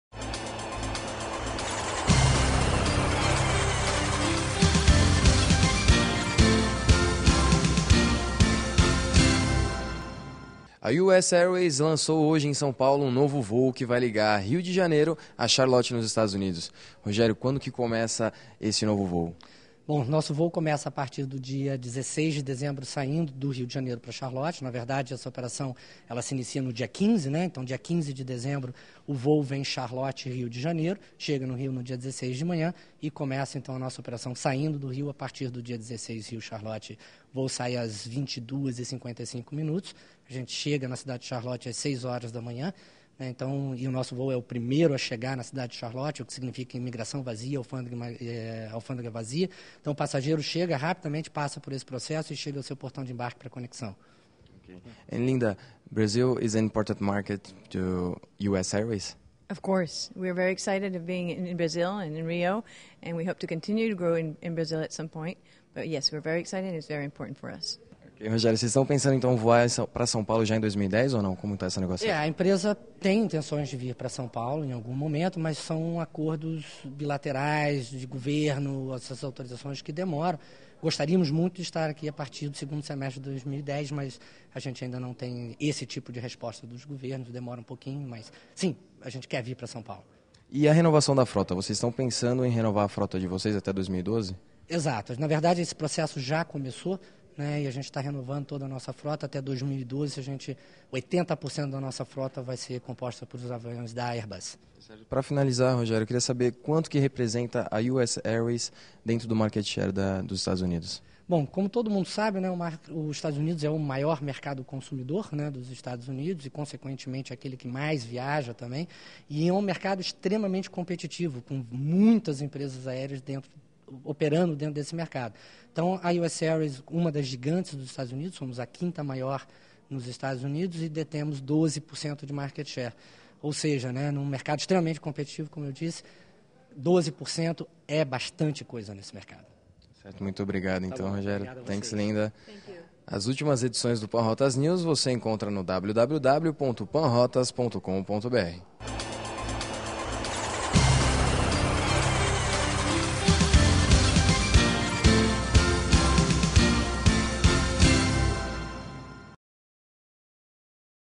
Publicidade US Airways apresenta voo Rio-Charlotte
A US Airways lançou, em São Paulo, seu primeiro voo no Brasil, que vai ligar, a partir de 16 de dezembro, o Rio de Janeiro a Charlotte, na Carolina do Norte (EUA). A cidade norte americana é um dos três hubs da companhia aérea  Filadélfia e Phoenix completam a lista. O voo diário e direto será a bordo de um Boeing 767 e terá duração de 8h45